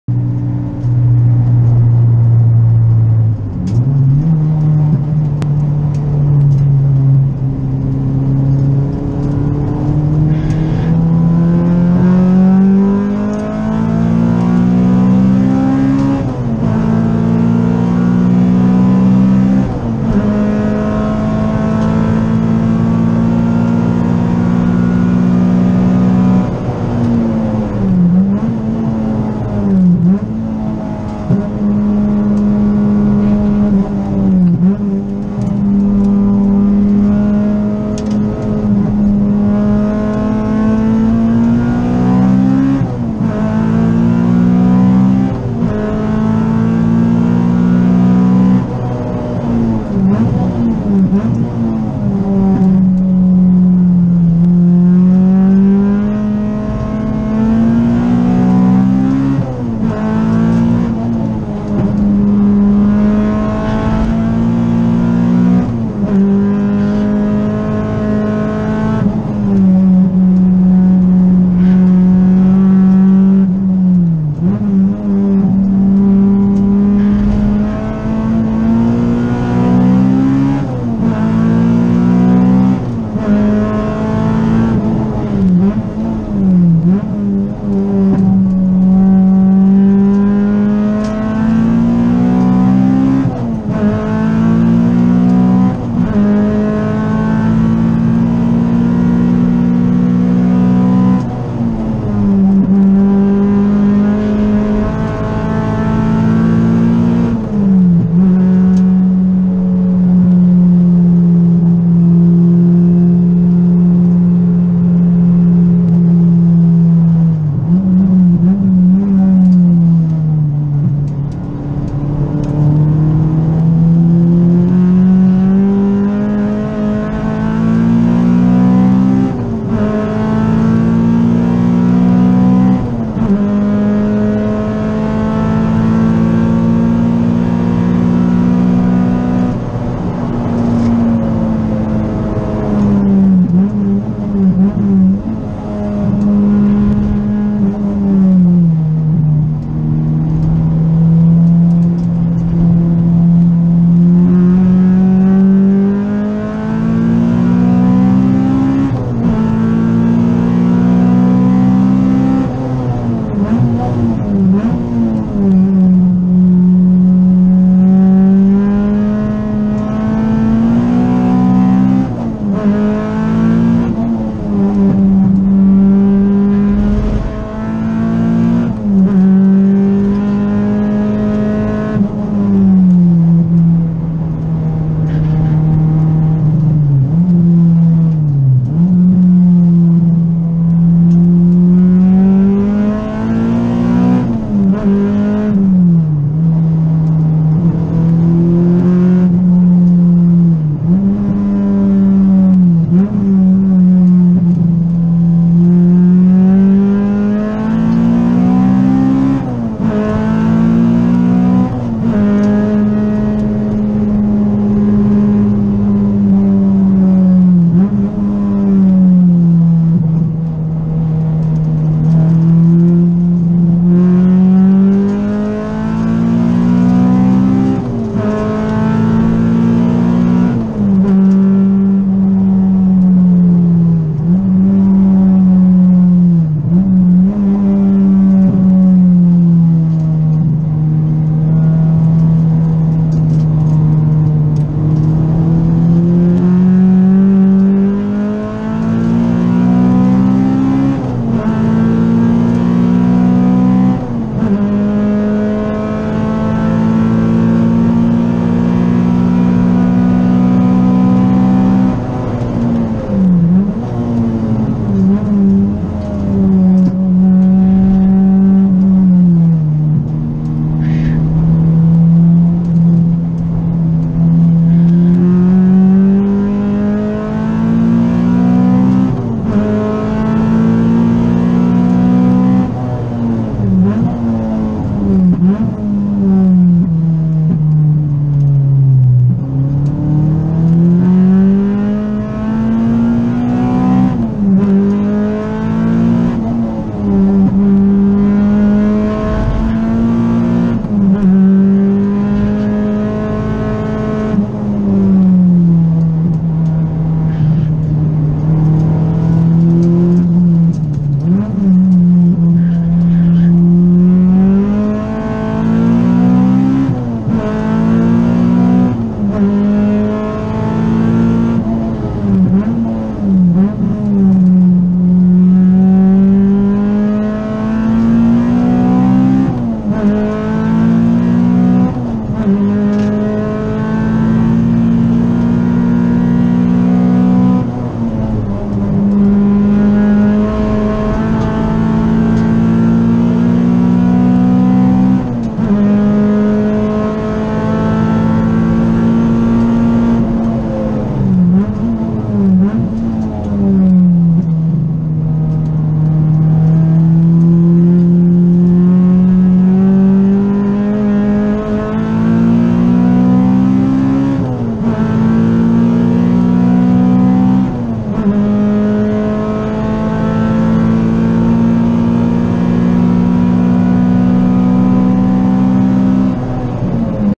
Taupo Drive Day May 2020 EG Civic B18CR Session 2 - 1
First Drive day post Covid pandemic at Bruce Mclaren Motorsport park. On a very cold track (11 degrees C air temp) and worn out front tyres.